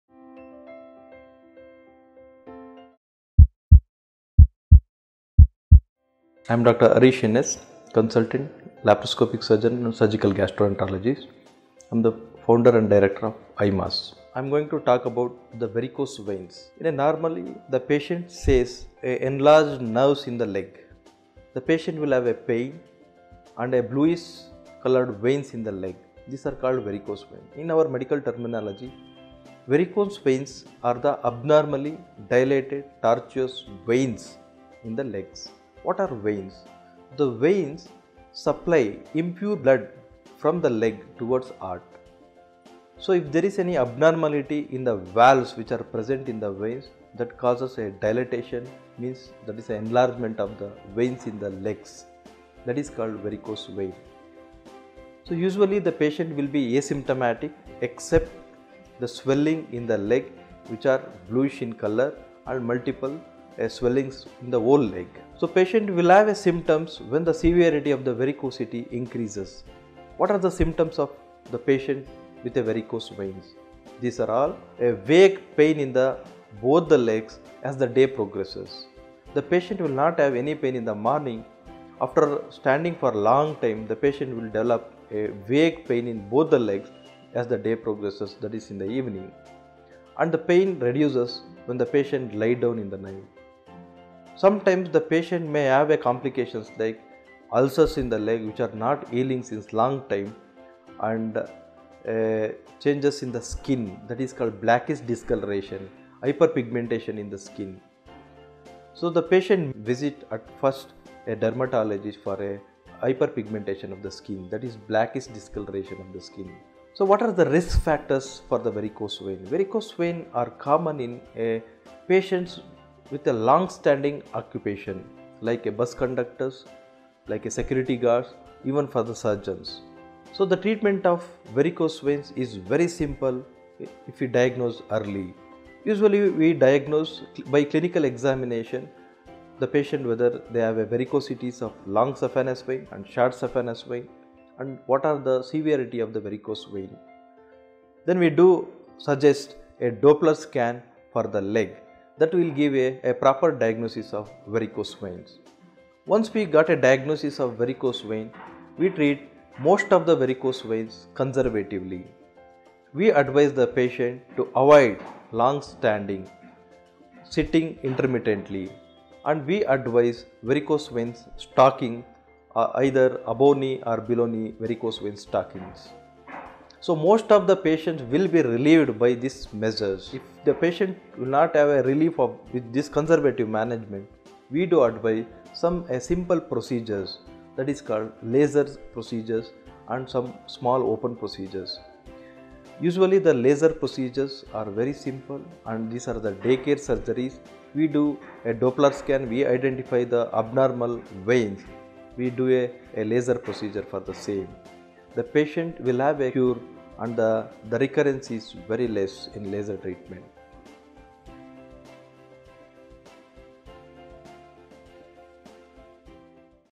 Varicose Veins: Symptoms, Risk Factors, Diagnosis, Treatment - Dr. Harish NS | Doctors' Circle
Dr. Harish NS | Appointment Booking No.8105498008
Laparoscopic Surgeon & Surgical Gastroenterologist|Prime Laparoscopy Clinic, Bangalore
Normally patients say enlarged veins in the legs . The patient will say of  pain and a  bluish coloured discoloration. This is called as Varicose Veins.  In medical terminology Varicose Veins are the abnormally dilated  tortuous  veins in the legs.  What are veins? Veins supply impure blood from the leg towards heart. So if there is any abnormality in the valves which are present , that causes  a dilatation that is enlargement if  veins in the legs that is called as Varicose Veins.  usually patient will be asymptomatic ,  the enlargement of  veins is  bluish  in  color and multiple swellings  in the  whole  leg. So the patient will have  symptom if the  severity of the varicosity increase.
What are the symptoms of the patient with Varicose Veins? The patient will have a vague  pain as the day  progresses. They will not have  any   pain in the morning. After standing for long duration, there is a vague pain in both the legs  ,  as  the day progresses  in the evening and the   pain reduces as  the patient  lies  down in the night. Sometimes  patient may have combinations like  ulcers  in the  leg  which are not healing for  a long time  and  changes in the skin, called as  blackish discoloration,  hyperpigmentation in the skin. So the patient visit a dermatologist for a hyperpigmentation of the skin, that is  blackish  discoloration. What are the risk factors for Varicose Veins?  Varicose Veins are common in patients with long standing occupation like bus conductors,  security guards, even  for  surgeons. So the treatment of Varicose Veins is very simple, if you diagnose  early. usually  we diagnose    by clinical  examination, the  patient whether they have  varicosity of long  sephanous vein , shirt sephanous  vein  and what are  the severity of   Varicose Veins. We do  suggest a Doppler  scan for the leg, that will give proper  diagnosis of  Varicose Veins. Once  we get  diagnosis, if we treat  them conservatively.  We  advice the patient to  avoid  long standing, sitting intermittently  and  we  advise  Varicose Veins  stocking  or either  above knee or below  knee  Varicose Veins  stockings.  Most of the patient will be relieved by such measures  if  patient will not be relieved  by conservatively  management, we advise  laser procedures and small open procedures.  Laser procedures are  day care  surgeries, we  do a Doppler  and  we identify  the  abnormal  veins, and we  do a  laser  procedure  the patient  will   have  cure and the  recurrency is  very less.